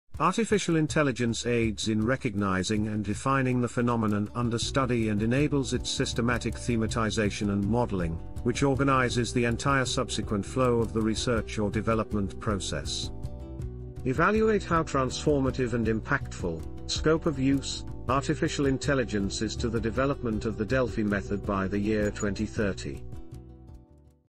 1. Phenomenon identification and modeling
Artificial Intelligence aids in recognizing and defining the phenomenon under study and enables its systematic thematization and modeling which organizes the entire subsequent flow of the research process.
Evaluate how transformative and impactful (scope of use) artificial intelligence is to the development of the Delphi method by the year 2030?